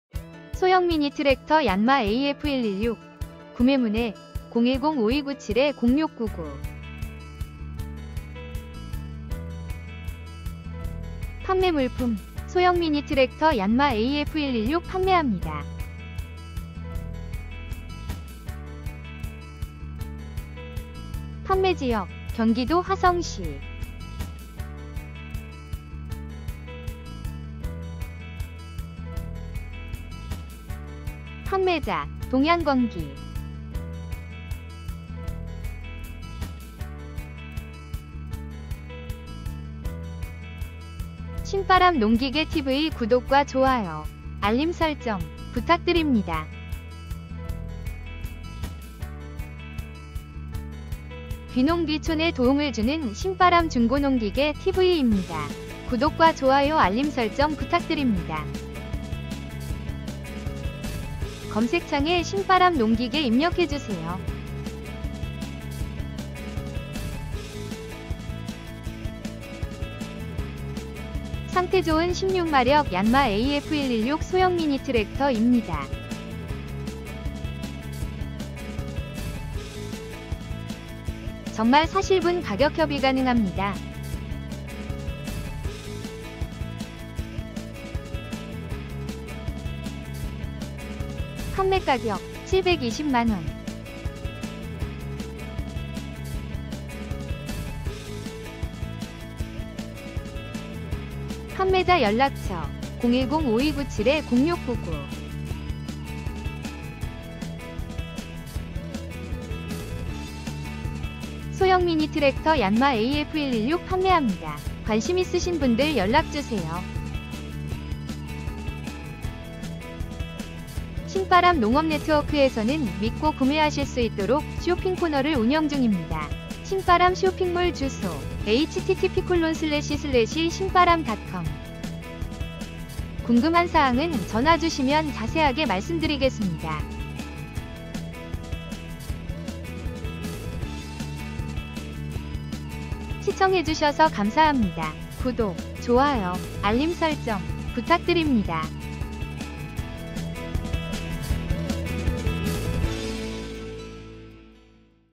[신바람 중고농기계밴드 동양건기 회원님] 소형미니트렉터 얀마AF-116 판매 - 중고트랙터 중고농기계 경운기 관리기 스키로더 굴삭기 화물트럭 매매 직거래 장터 사이트
판매물품 : 소형미니트렉터 얀AF-116 (16마력)

판매지역 : 경기도 화성시

판매가격 : 720만원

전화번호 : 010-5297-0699

기타 :상태좋은 16마력 얀마AF-116 소형미니트렉터 입니다
정말 사실분 가격협의가능합니다
각종 소형농기계 판매하고있으니 편하게 오셔서 시운전해보세요
친절상담해드립니다